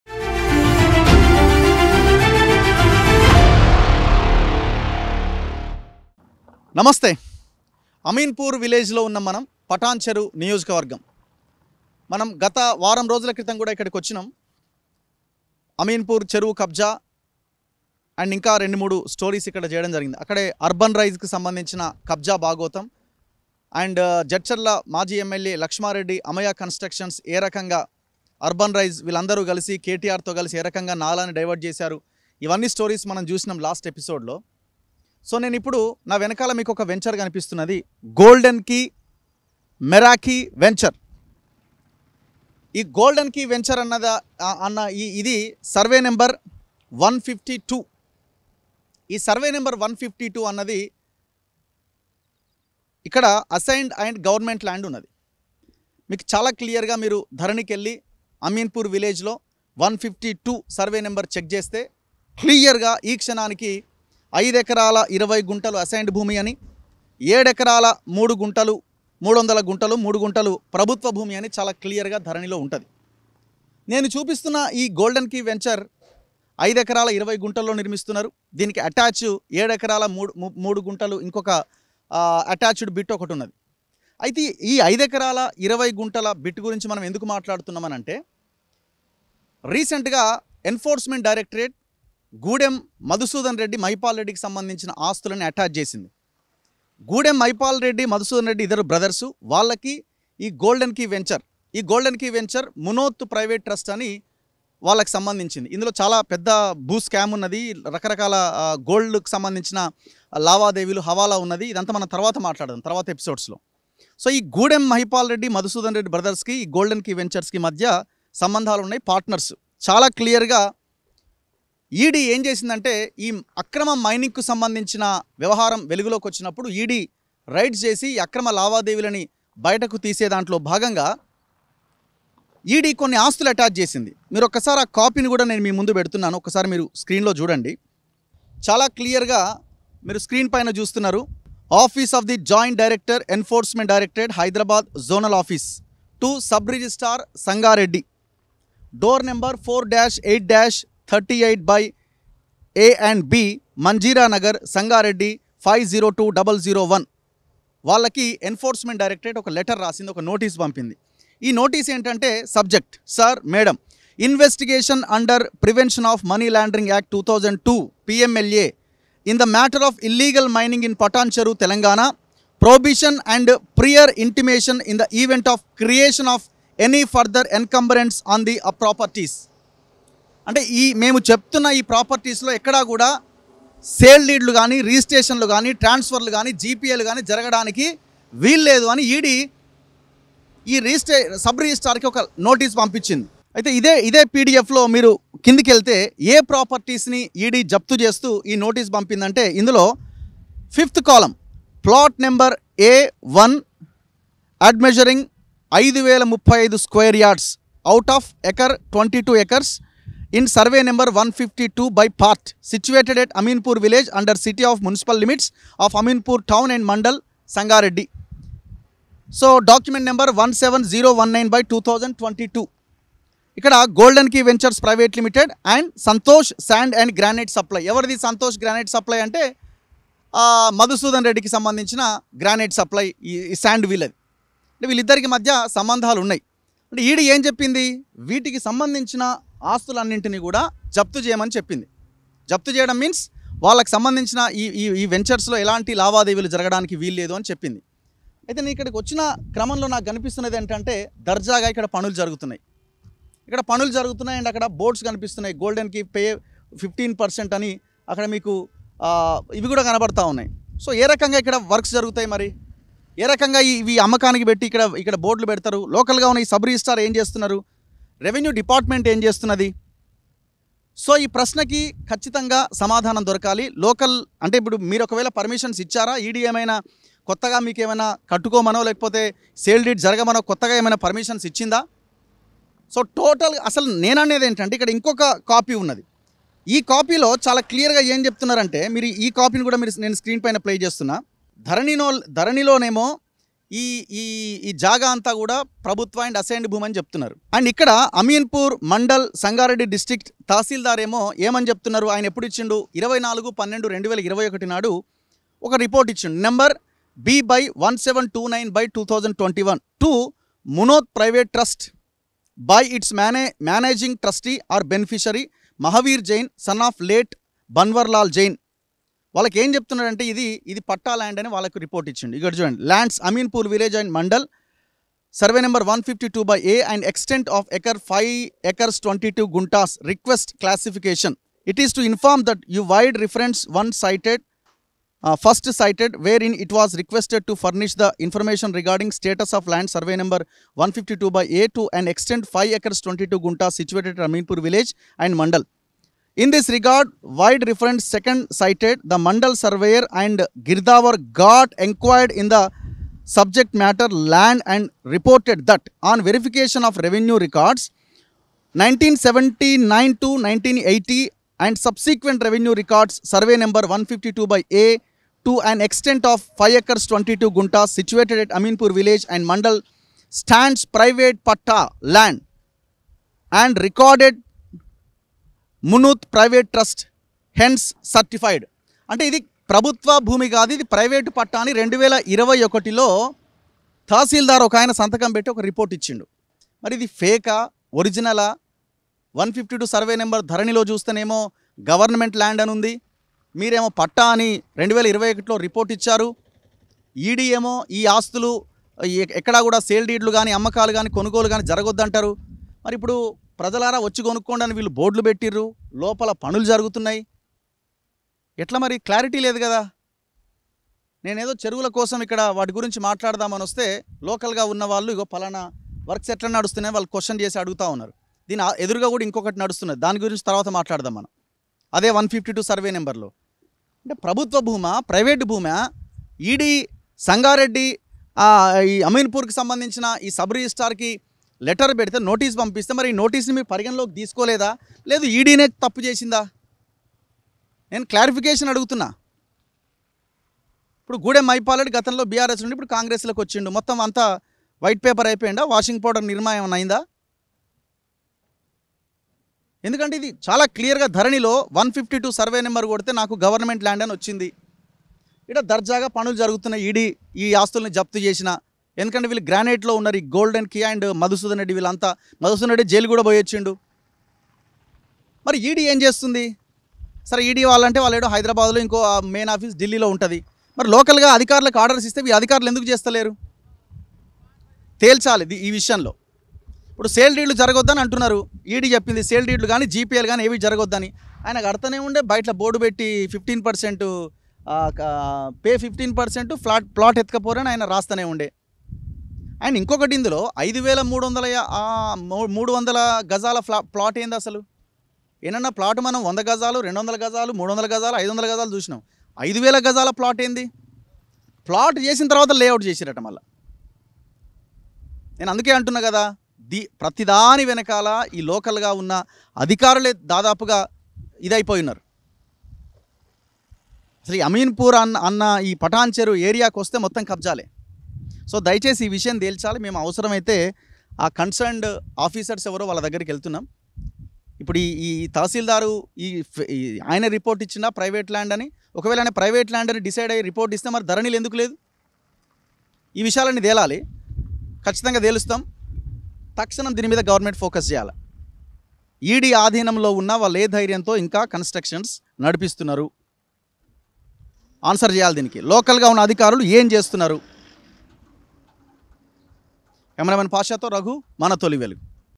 Ameenpur Goldenkey Meraki Scam : అమీన్ పూర్ లో ఈడీ గీడీ జాన్తా నై ! గోల్డెన్ కీ వెంచర్ అరాచకం!
Ameenpur Goldenkey Meraki Scam : అమీన్ పూర్ లో ఈడీ గీడీ జాన్తా నై ! గోల్డెన్ కీ వెంచర్ అరాచకం! | ManaTolivelugu
Goldenkey Meraki in Ameenpur, Hyderabad
Goldenkey Meraki Bachupally, Hyderabad